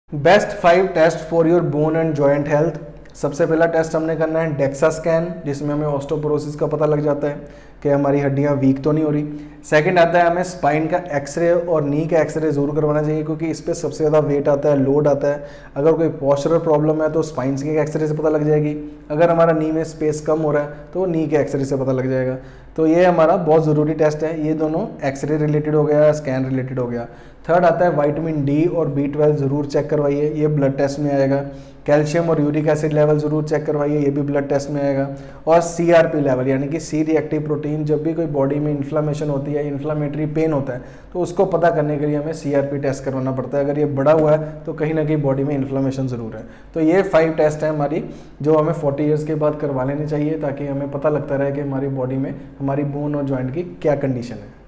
Best 5 Test to Check Bone Health, Osteoporosis, How to check Bone & Joint Health, Vitamin D, Calcium
5 Best Test to Check Bone and Joint Health After 40 Years Of Age